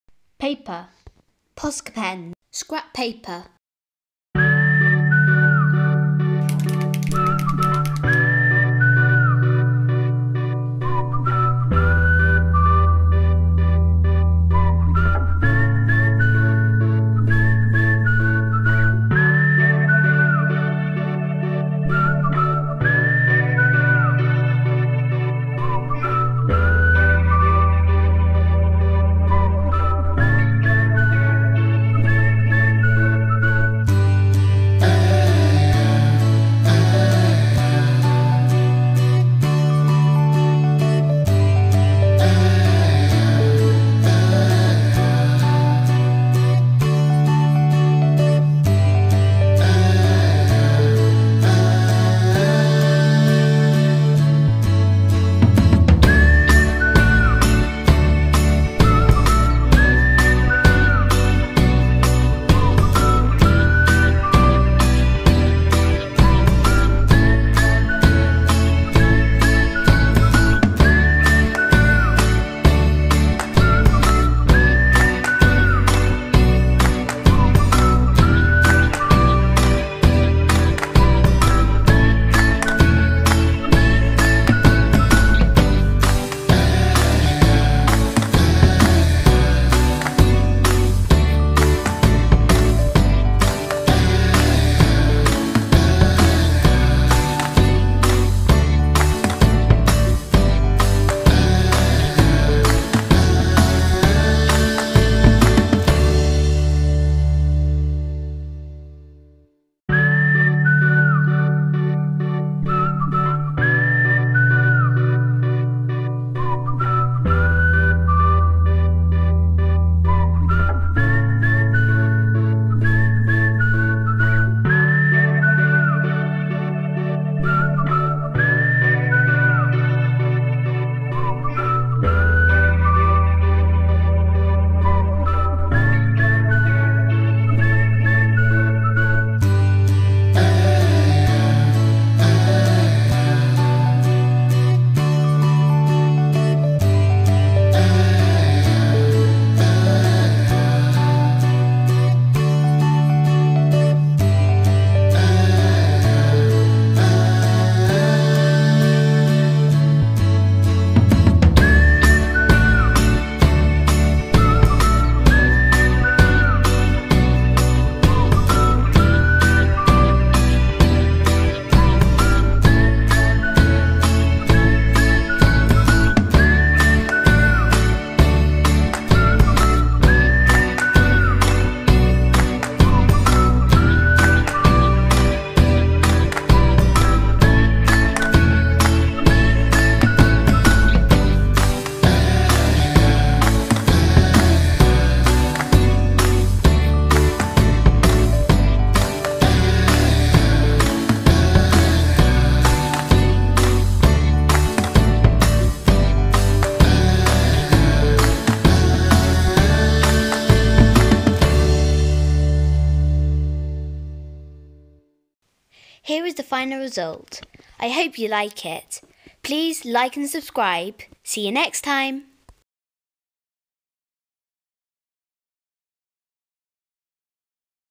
Drippy art (first video 😁😎😎)
You will need:
Posca pens ✅
(Coloured) paper ✅
Scrap paper ✅
Apron (optional)
Table covering (recommended even though I didn't have it 😬)